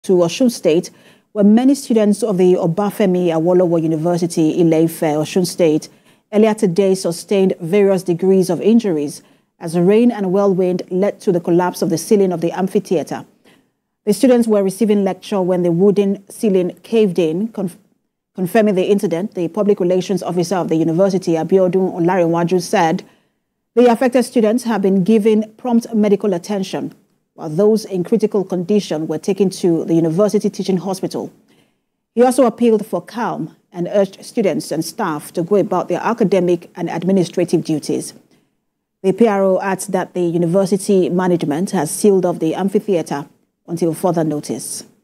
OAU Students Injured As Amphitheatre Ceiling Collapses
many students of the Obafemi Awolowo University, Ile-Ife, Osun State earlier today sustained various degrees of  injuries as rain and whirlwind led to the collapse of theceiling of the amphitheater.